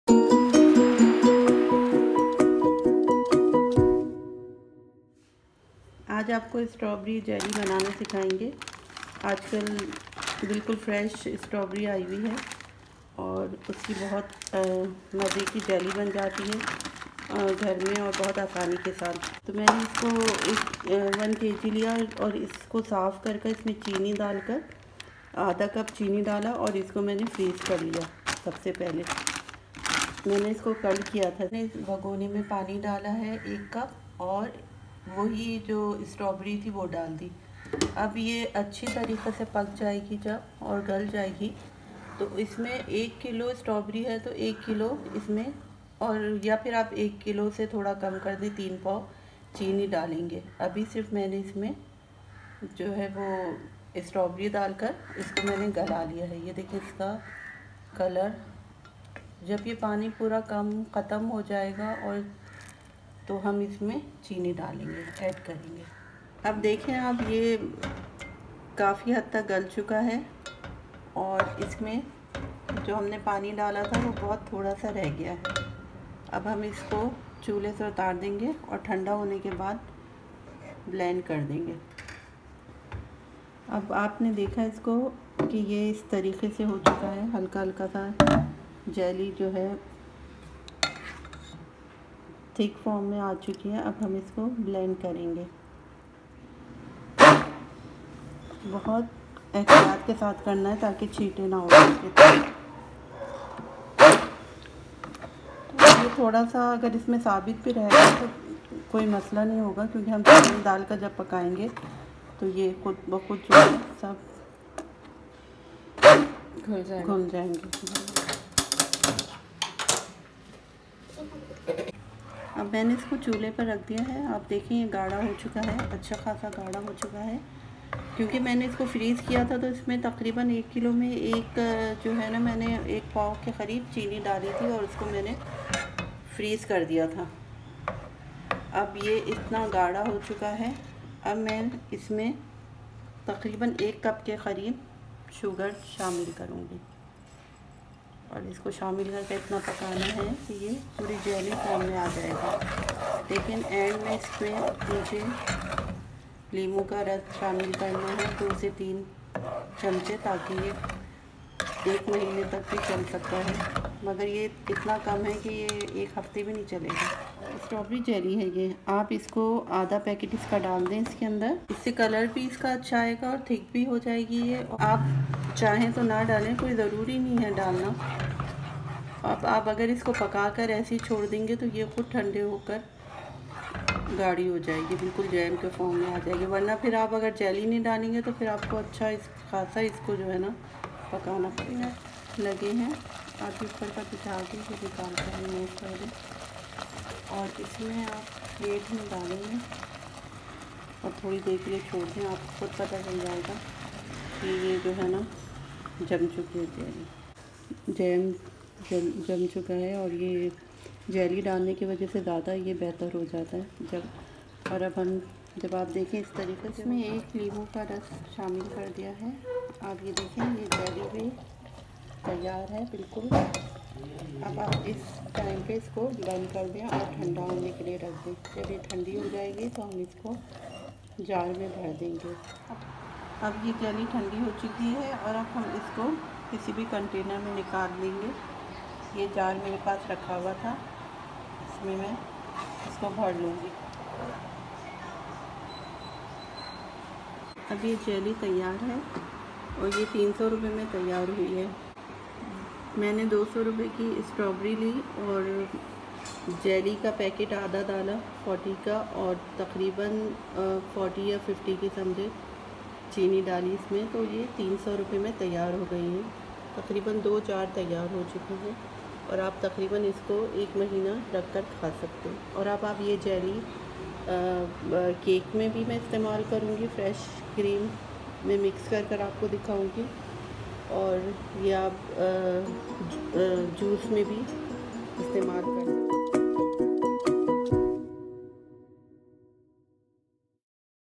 Easy Strawberry Jam Recipe || 4 ingredients recipe || Homemade Jam in Rs.300 || Nano Ammi’s Kitchen
Welcome back to Nano Ammi’s Kitchen..

“Strawberry Jam Recipe”
Ingredients:
🍓 Strawberries 1 kg
🍓 Sugar 3 pao
🍓 Jelly powder 1/2 packet
🍓 Lemon 1

Aj bohat hi meethi aur mazaydar recipe lay k ai hun main jo k sirf Rs. 300 ya ussy bhi kum mai ban jayegi!

Umeed karti hun ap log ko pasand ayegi!!